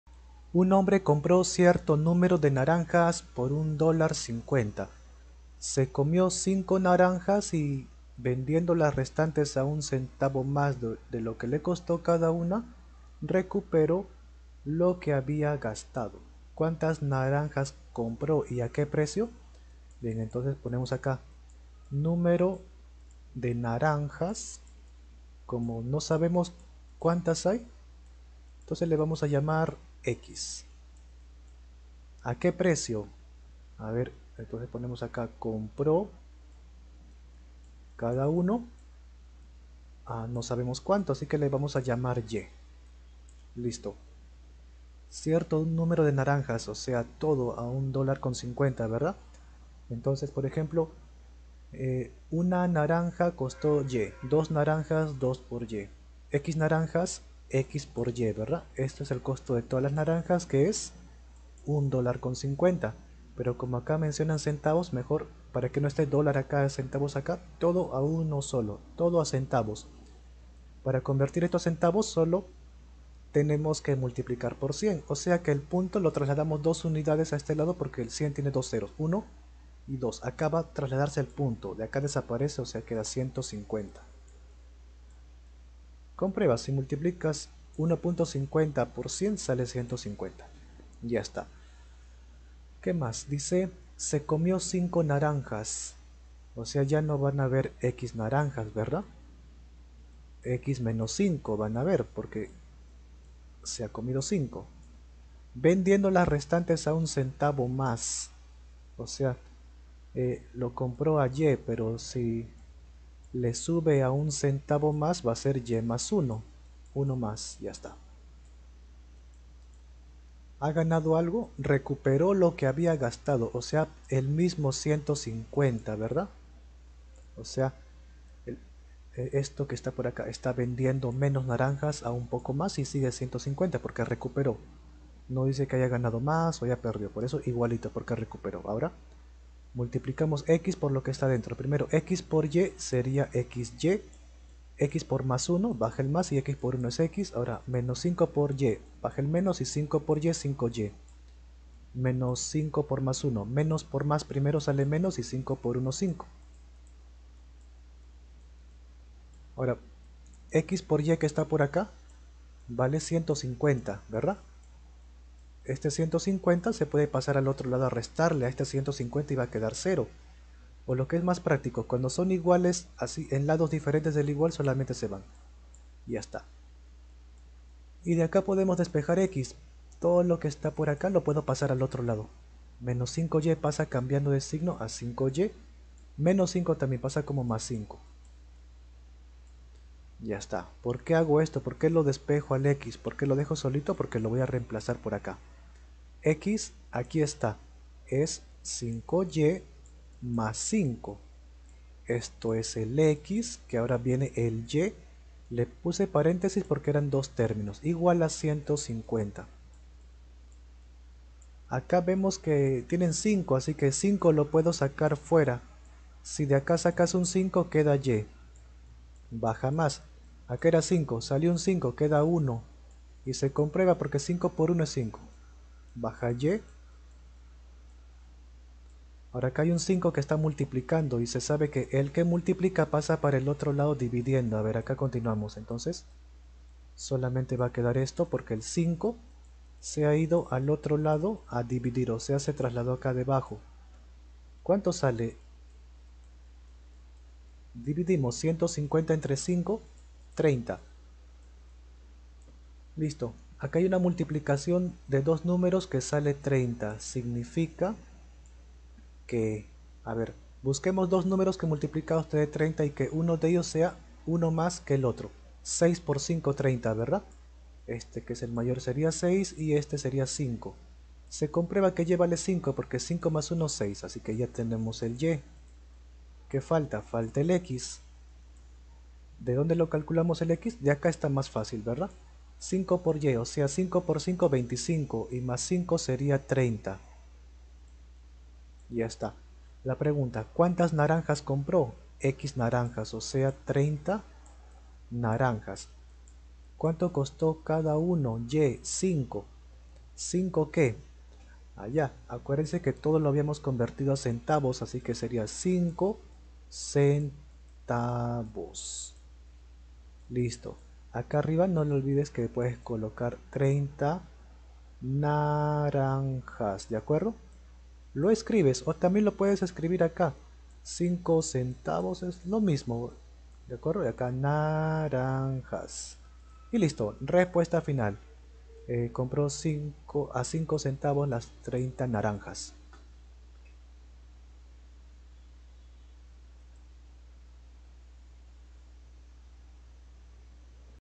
Un hombre compró cierto número de naranjas por $ 1.50 Se comió 5 naranjas y vendiendo las restantes
Un hombre compró cierto número de naranjas por $ 1.50 . Se comió 5 naranjas y vendiendo las restantes a 1 ctvo . más de lo que le costó cada una recuperó lo que había gastado. ¿Cuántas naranjas compró y a qué precio?